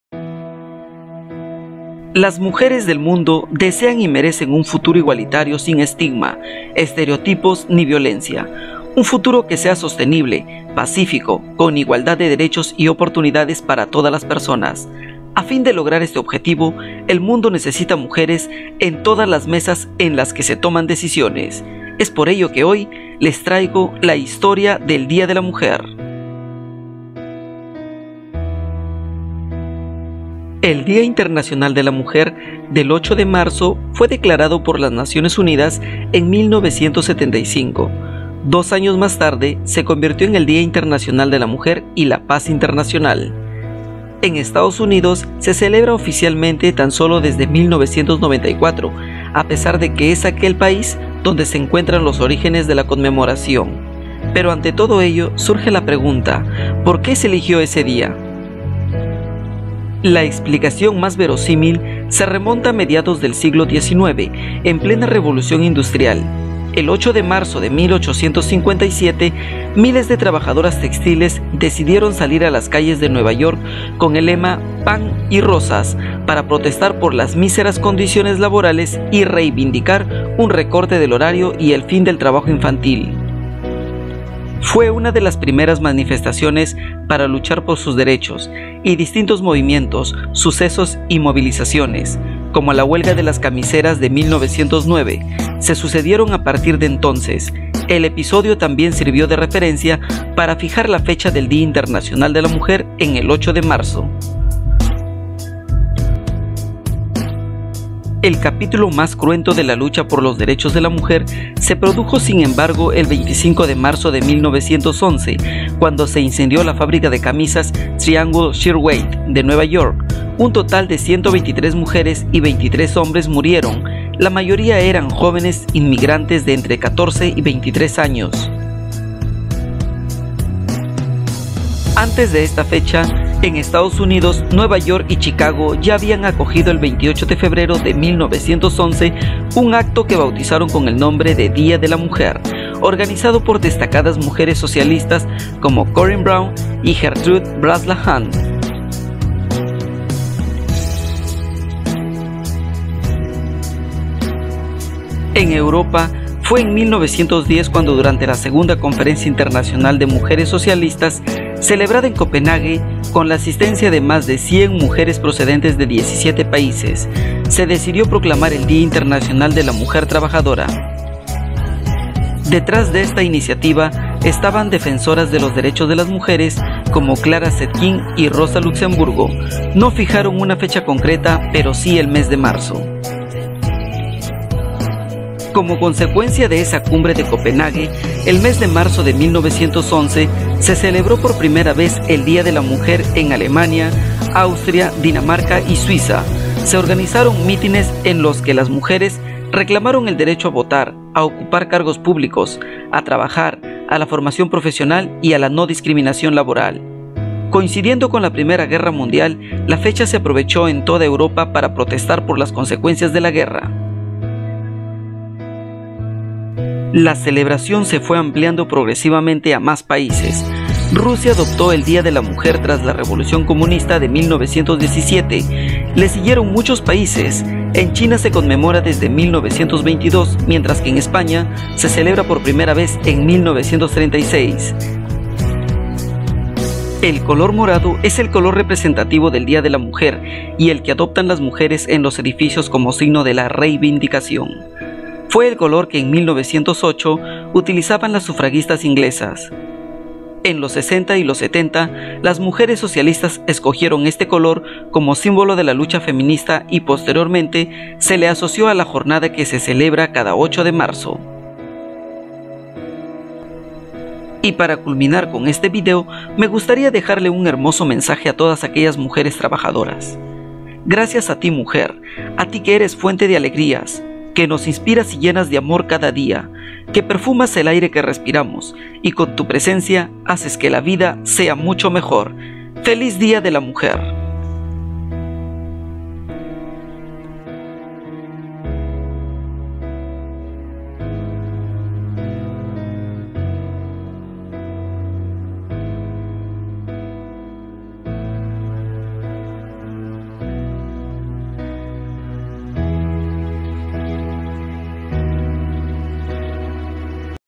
👩‍🦰 DÍA INTERNACIONAL DE LA MUJER ¿Qué pasó el 8 de marzo de 1857?😱
El Día Internacional de la Mujer del 8 de marzo fue declarado por las Naciones Unidas en 1975. Dos años más tarde se convirtió en el Día Internacional de la Mujer y la Paz Internacional. En Estados Unidos se celebra oficialmente tan solo desde 1994, a pesar de que es en aquel país donde se encuentran los orígenes de la conmemoración. ¿Por qué se eligió ese día?